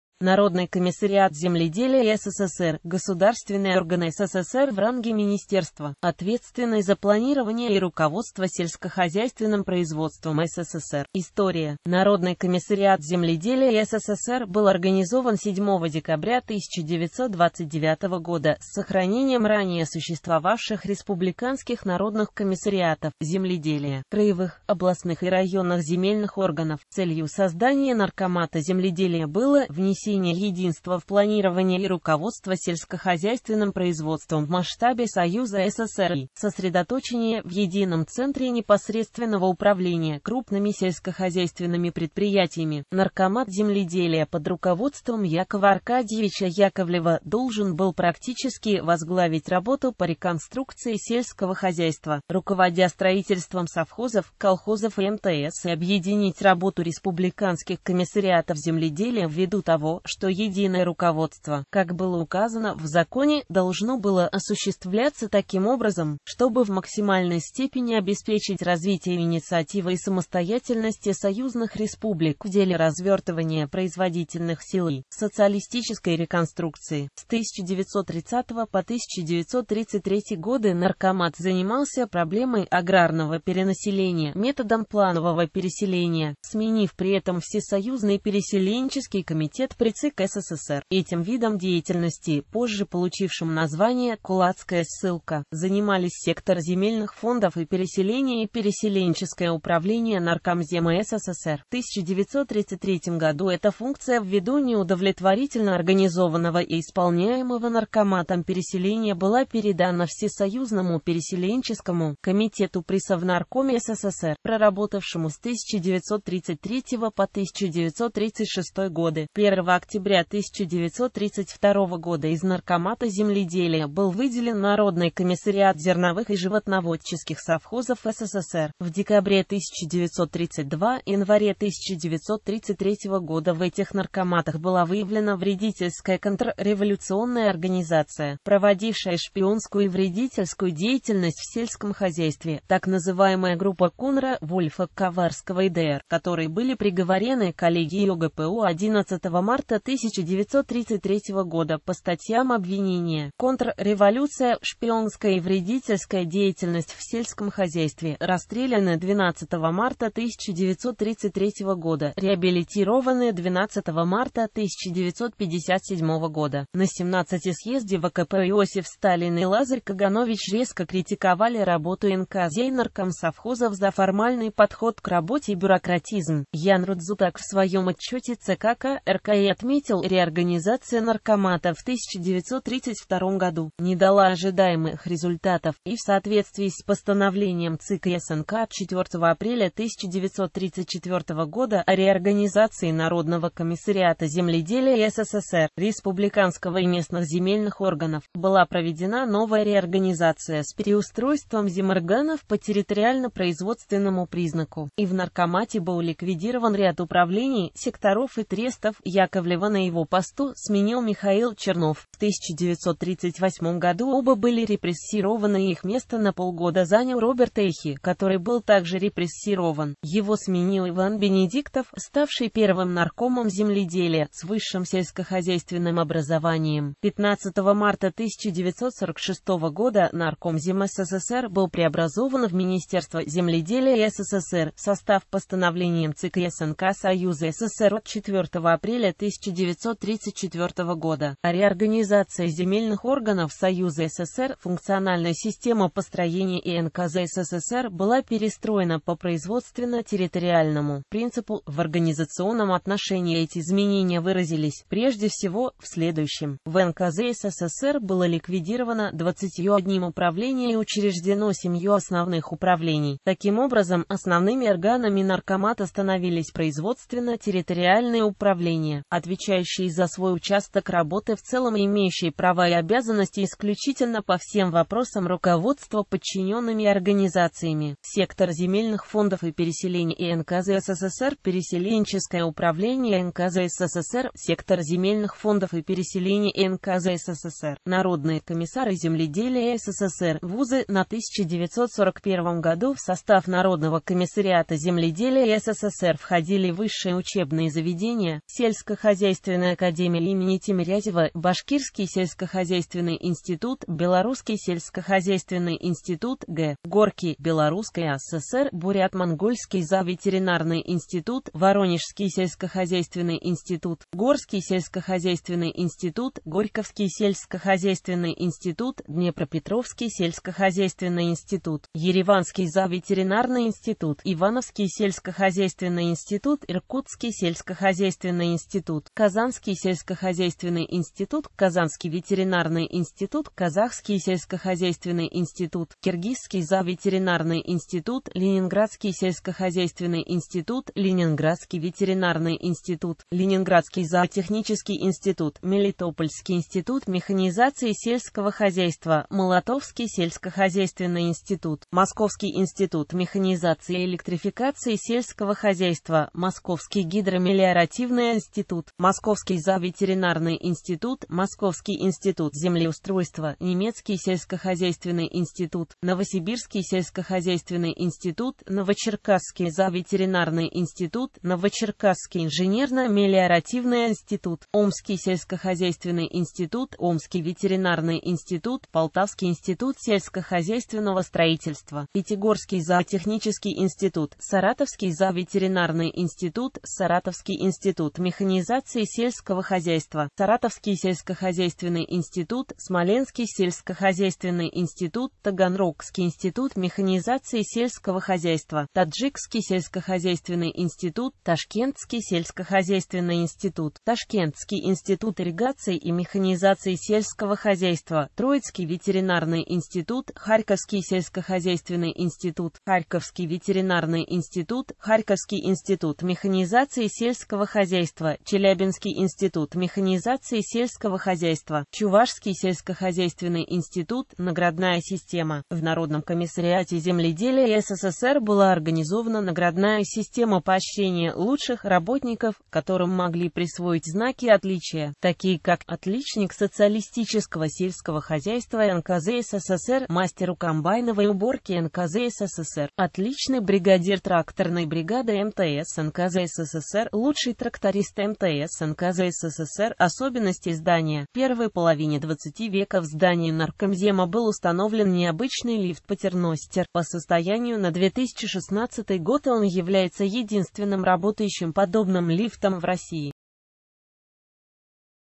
Народный комиссариат земледелия СССР
Народный комиссариат земледелия СССР — государственный орган СССР в ранге министерства, ответственный за планирование и руководство сельскохозяйственным производством СССР.